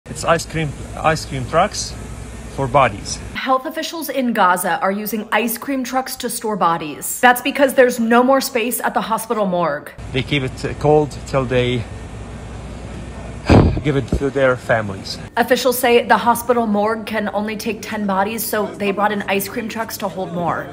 Ice cream truck used as makeshift morgue
Health officials are using icecream trucks to store the bodies of dead Palestinians because there is not enough space at a hospital morgue in the central Gazan town of Deir Al-Balah.